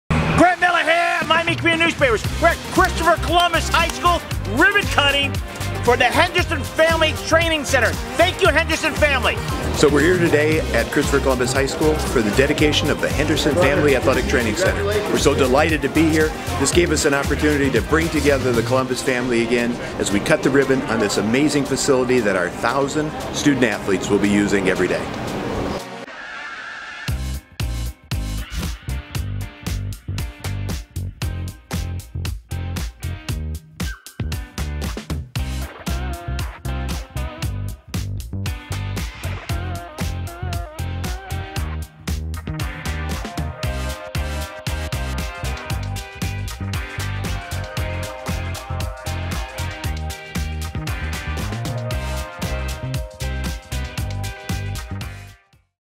Grant Miller attends the Ribbon Cutting of the Henderson Family Athletic Training Center
Grant Miller attends the Ribbon Cutting of the Henderson Family Athletic Training Center made possible by the generous donation of NFL Cornerback and former Christopher Columbus High School Alumni, C.J. Henderson!